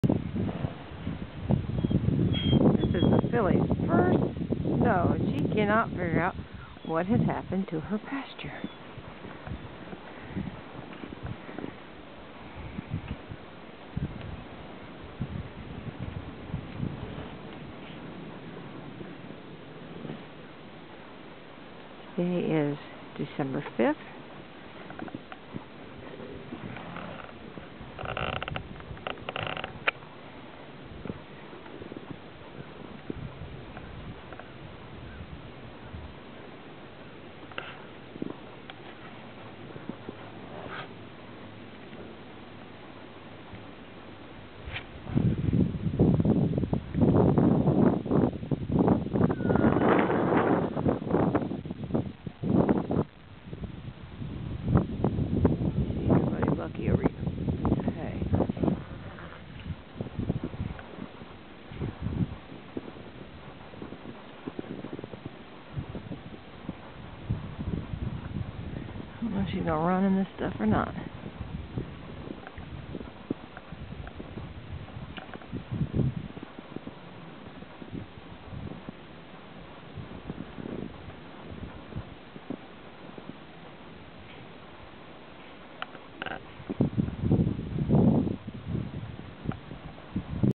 Brit Jet Boogie's first snow
Brit Jet Boogie, a 7 month old Appaloosa filly, sees snow for the first time.